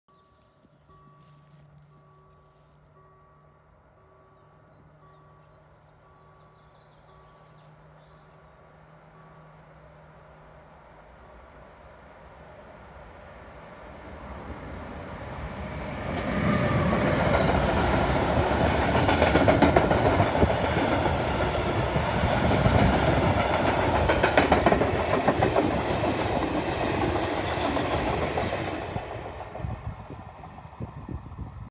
Short Mixed Freight Train With DB-Cargo Locomotive 189 086-2 at Breyell Germany 12.6.2022 🎥👍👍👍👍👍👍👍👍🚂
MixedFreightTrain ShortTrain ShortLines Train go to Cologne Gremberg Germany ! Trainspotting at Breyell Germany , June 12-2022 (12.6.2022)